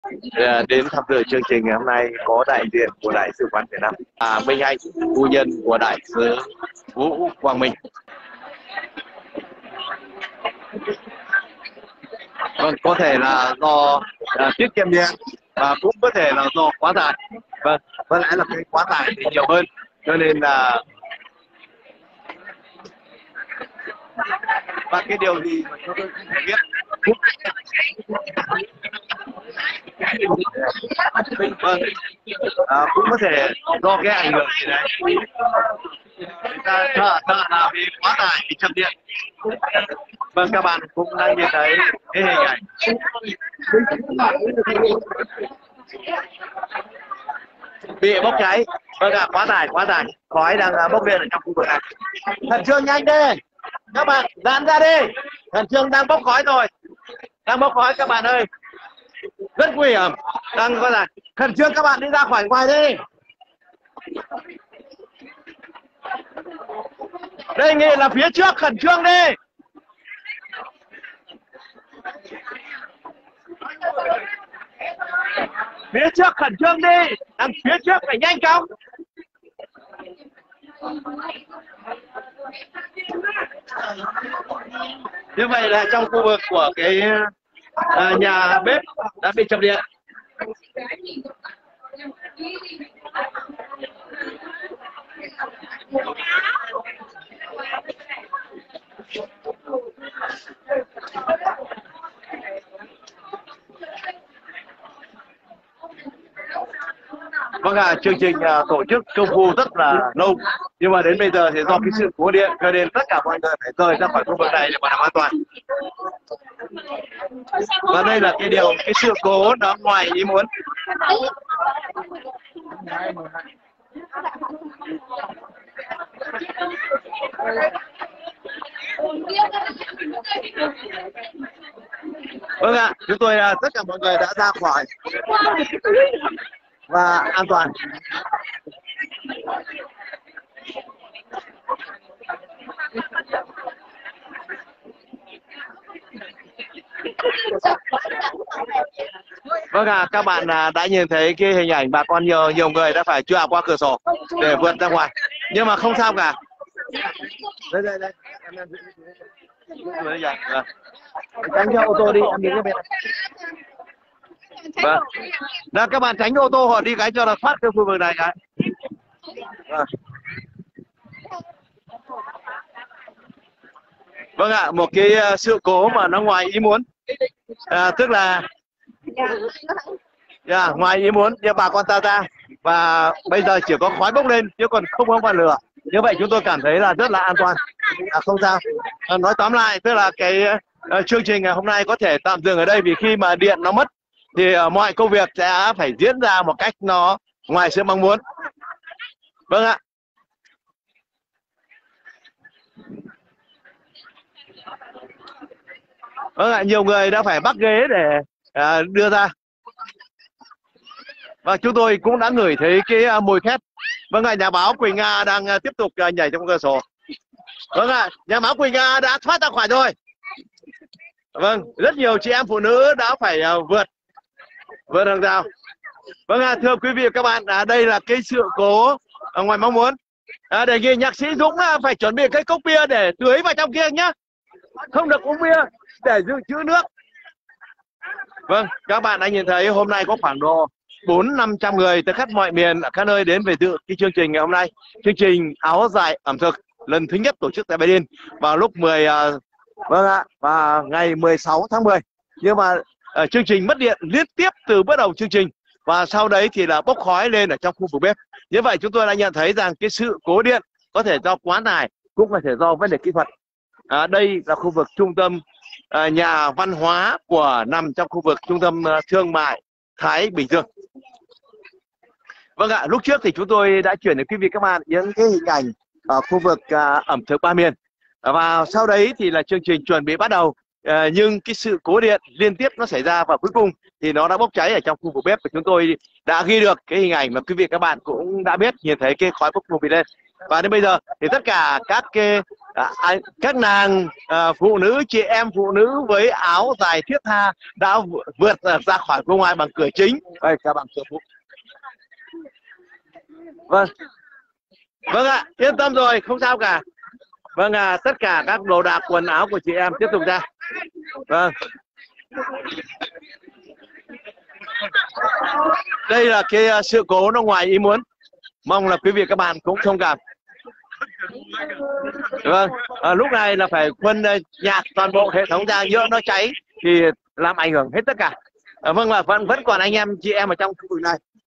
Phu nhân Đại sứ Việt Nam tại Đức tháo chạy khỏi „Áo dài và ẩm thực“ ở Berlin!
Hội trường đang sôi nổi thì đột ngột mất điện, khói mờ mịt. Phu nhân  Đại sứ cùng hàng trăm người tháo chạy qua một hành lang hẹp và tối.
Nhiều người quá hoảng loạn, vội leo qua cửa sổ thoát thân

Sự việc xảy ra chiều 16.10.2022, tại Trung tâm thương mại Thái Bình Dương của người Việt tại Marzahner Str. 17 13053 Berlin.